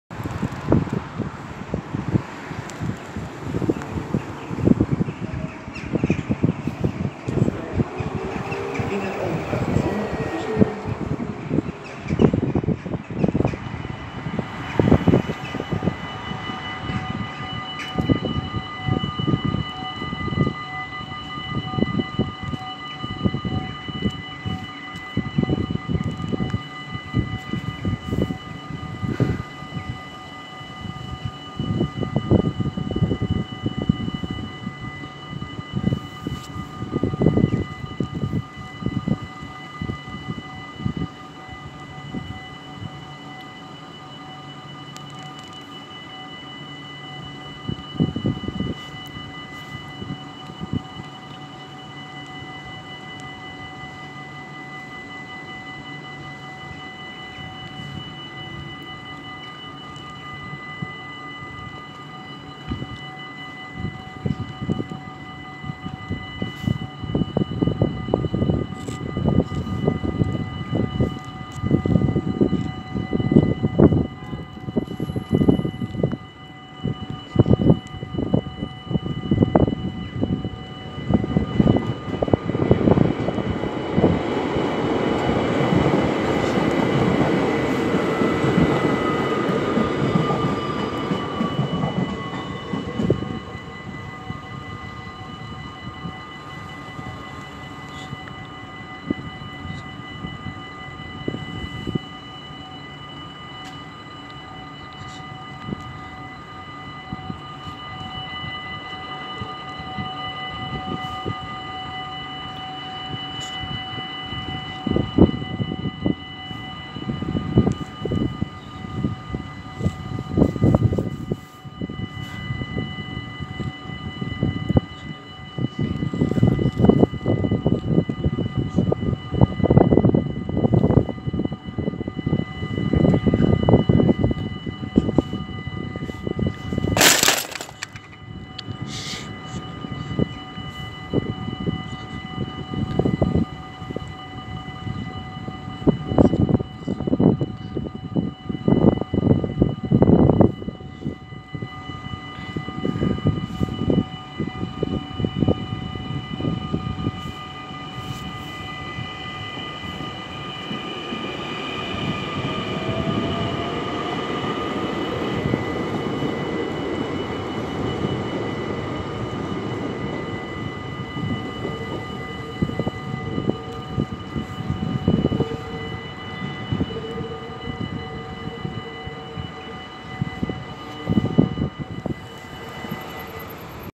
Spoorwegovergang Veenendaal // Dutch railroad crossing
Locatie: Kerkewijk
Traject: Utrecht - Rhenen
Wacht borden: 14
Rode lichten: 28
Andreaskruisen: 10
Bellen: 10
Bomen: 10
Passeren:
SLT als SPR van Rhenen naar Breukelen
SNG als SPR van Breukelen naar Rhenen